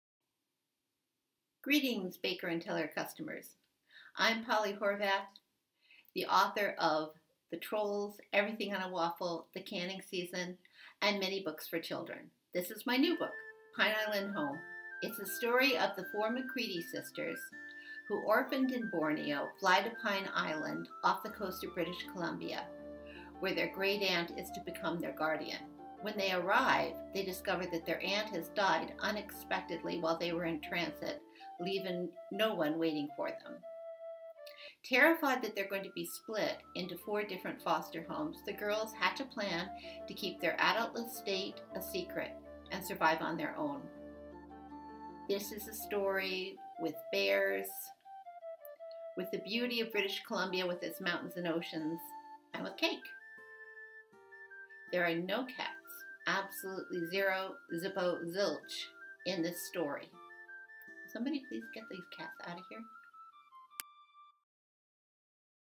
Pine Island Home: A preview from Polly Horvath
When the McCready sisters' parents are washed away in a tsunami, their Great Aunt Martha volunteers to have them live with her on her farm in British Columbia. But while they are traveling there, Martha dies unexpectedly, forcing Fiona, the eldest, to come up with a scheme to keep social services from separating the girls - a scheme that will only work if no one knows they are living on their own.